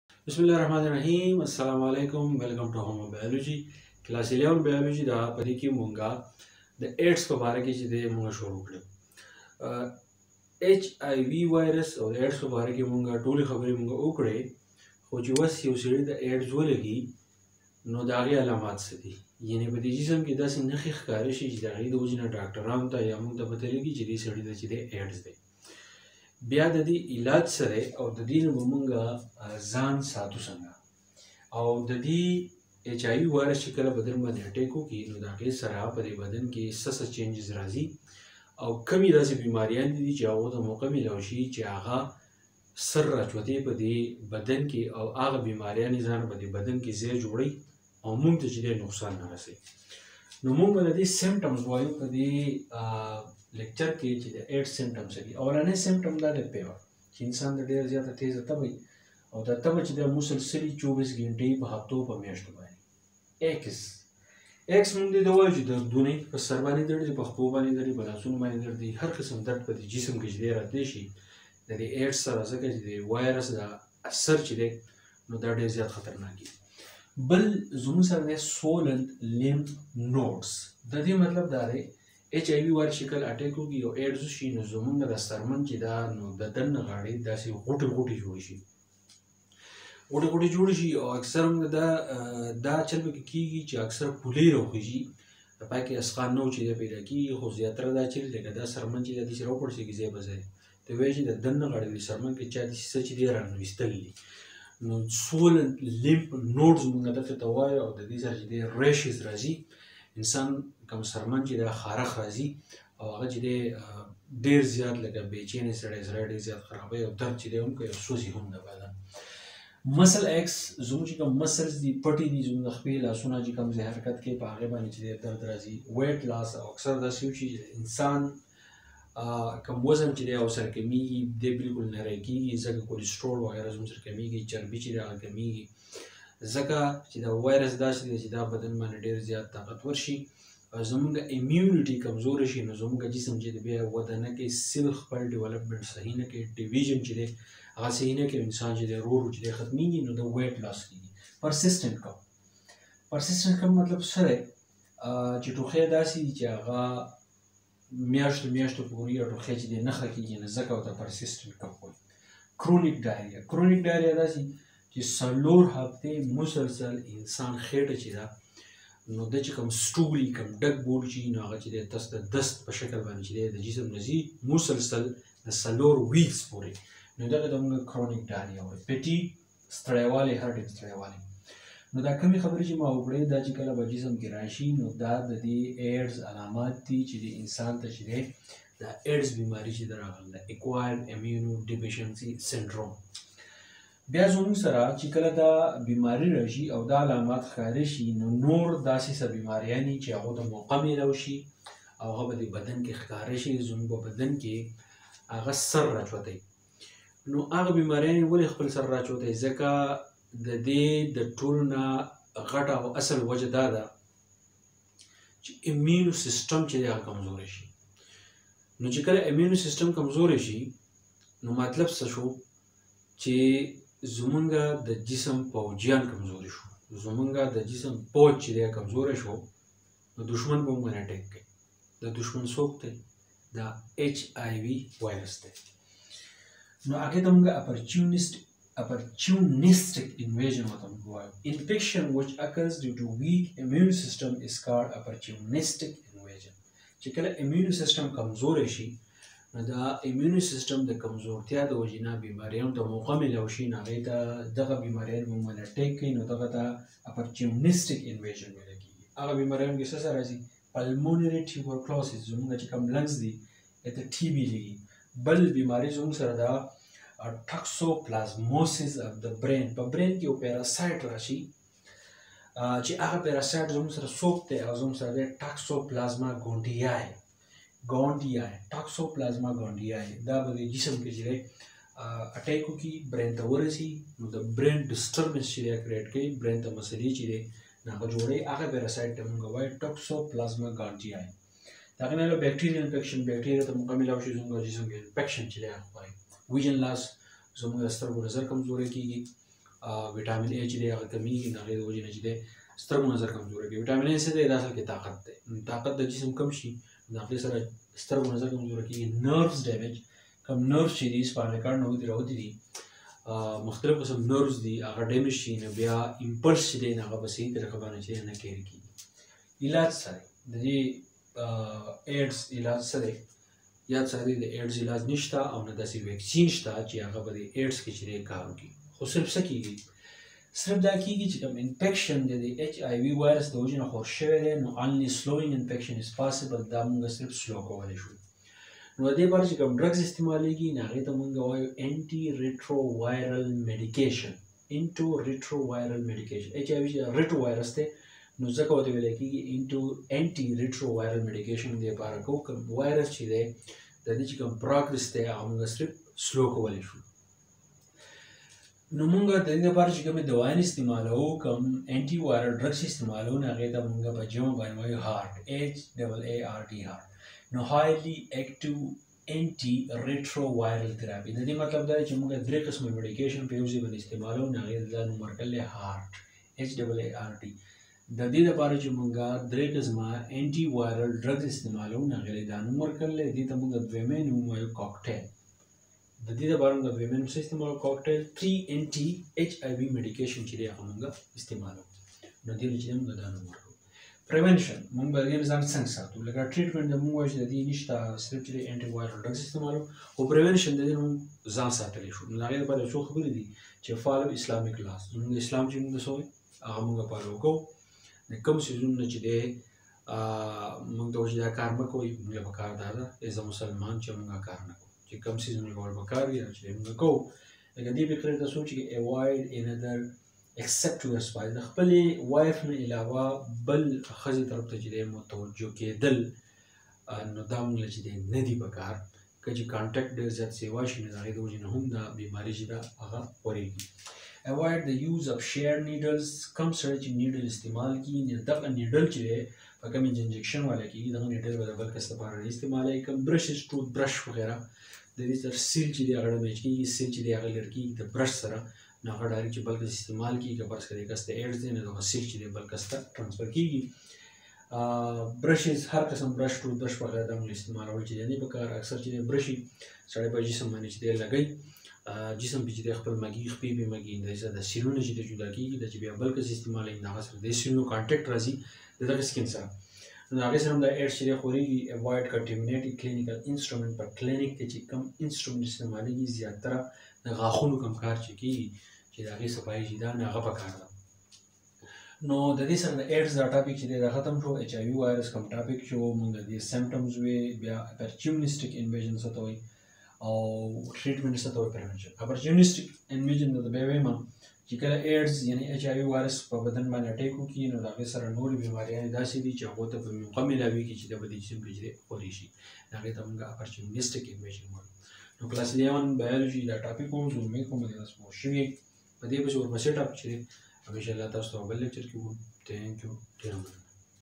Symptoms of AIDS | Opportunistic invasion | HAART | 11th biology
In this lecture you will learn about symptoms of aids, opportunistic invasion, treatment of aids, Haart, cocktail and prevention of aids in pashto from 11th class biology